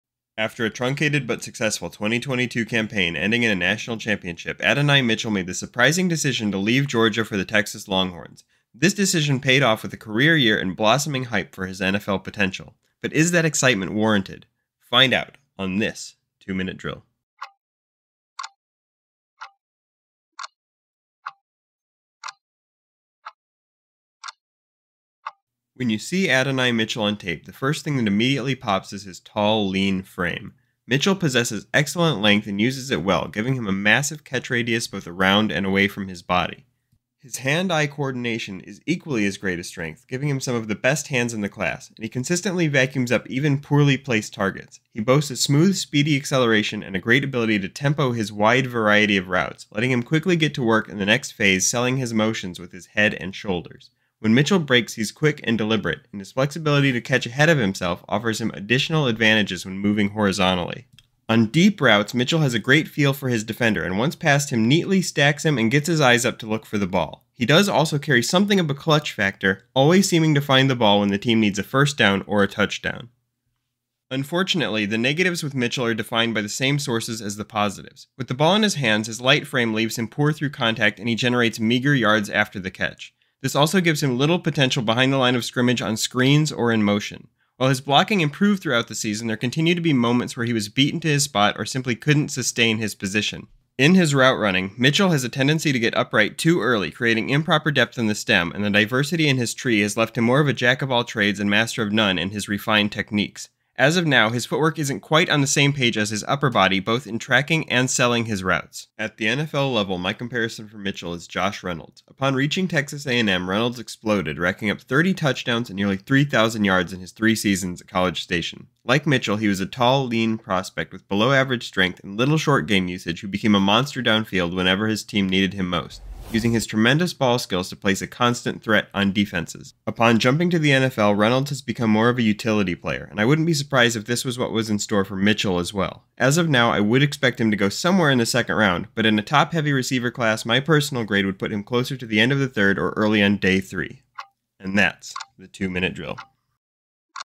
Adonai Mitchell is set to TAKE OFF! 🚀 | 2024 NFL Draft Profile & Scouting Report
AD Mitchell made his presence felt with both Georgia and Texas and now heads to the NFL hoping to do the same. How does he compare to the other top wideouts of this class like Marvin Harrison Jr, Malik Nabers, Rome Odunze, Keon Coleman, Xavier Worthy, and others?

Join our NFL Draft analysis team as we highlight Mitchell's strengths and weaknesses and breakdown his game for the next level